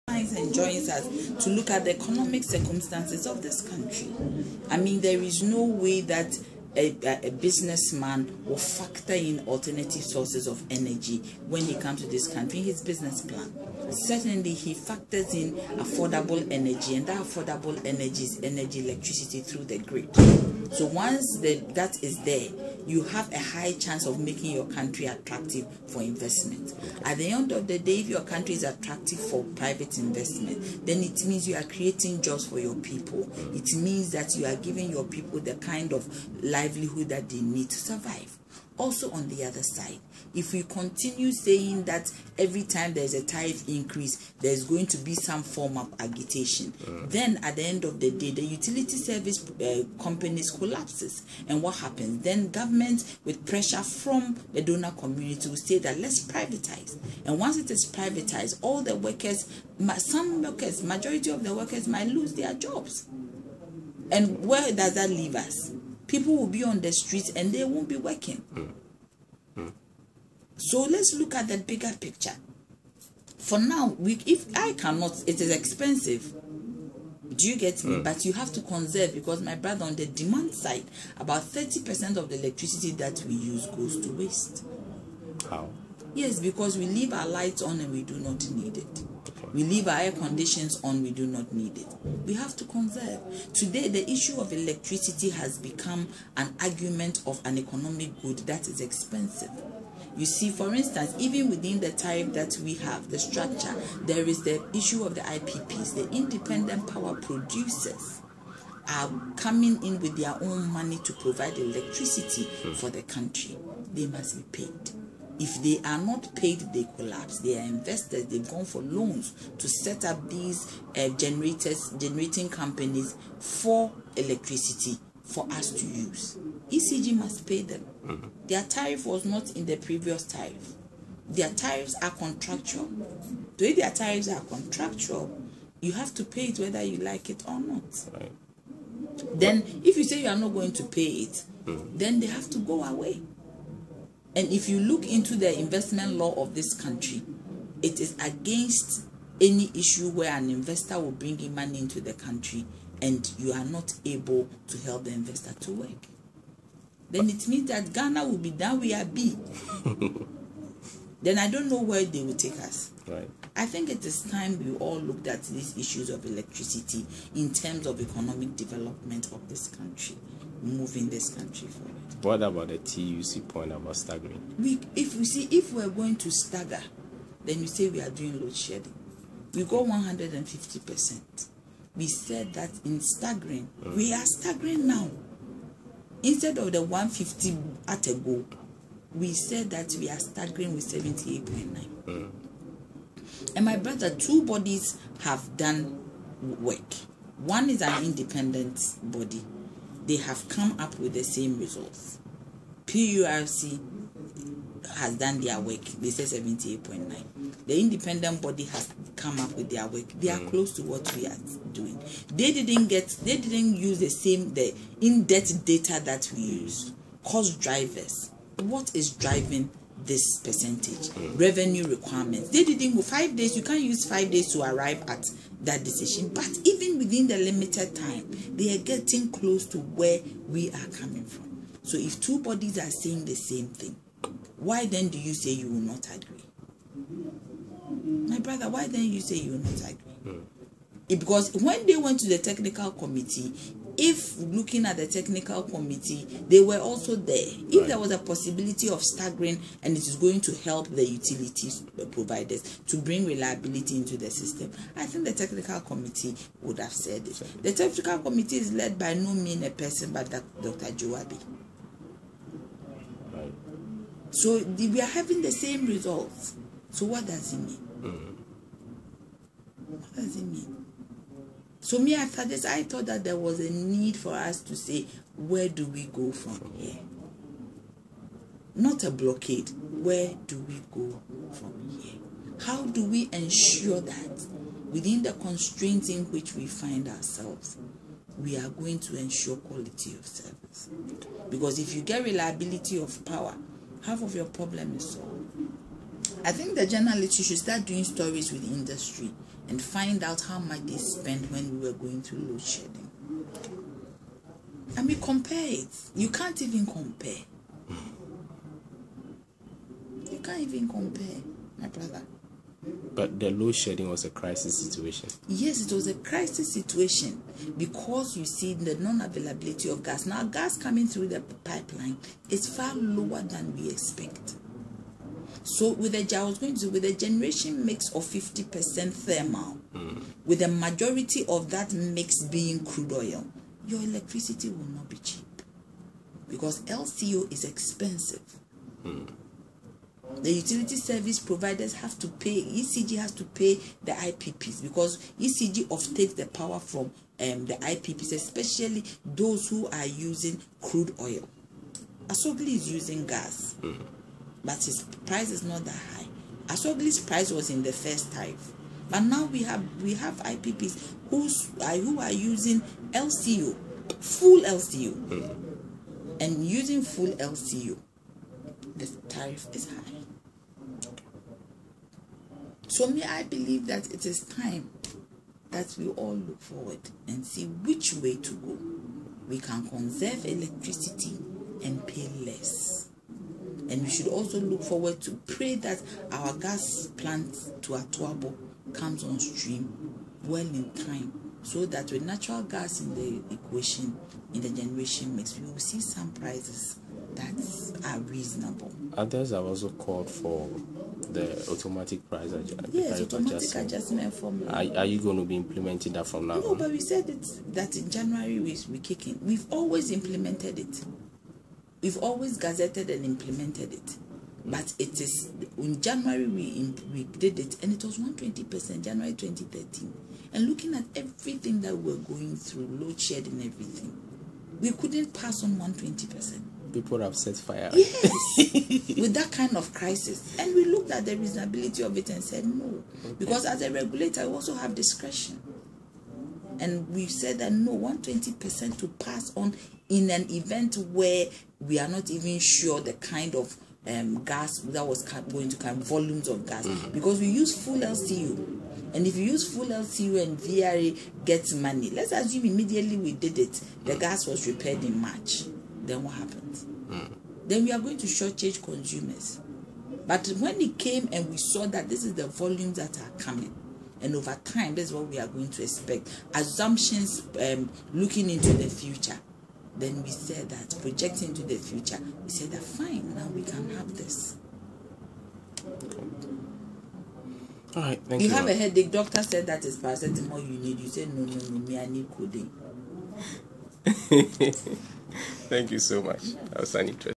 Nana Yaa Jantuah of PURC on Utility Tariff increases
Nana Yaa Jantuah of PURC justifies Utility Tariff increases